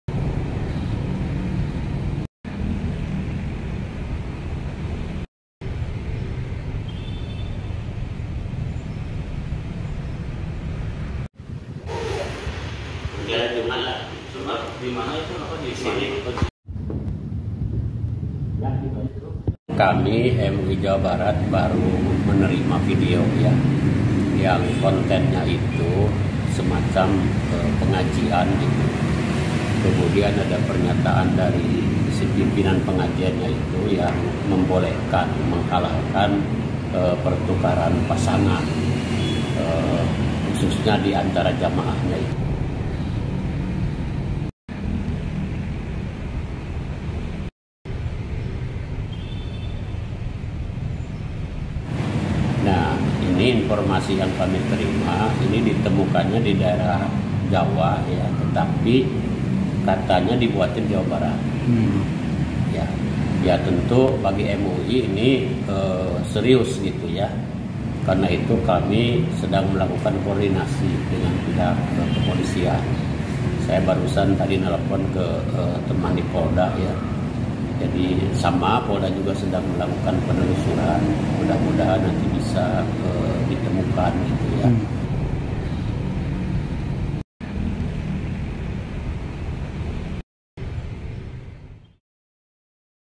Video Aliran Sesat Boleh Tukar Pasangan, MUI Jabar Lakukan Penelusuran | Liputan6
LIPUTAN6.COM - Video Aliran Sesat Boleh Tukar Pasangan, MUI Jabar Lakukan Penelusuran | Liputan6

Majelis Ulama Indonesia (MUI) Jawa Barat menyatakan bakal turun tangan menyelidiki video viral aliran sesat yang memperbolehkan pertukaran pasangan antar jamaah.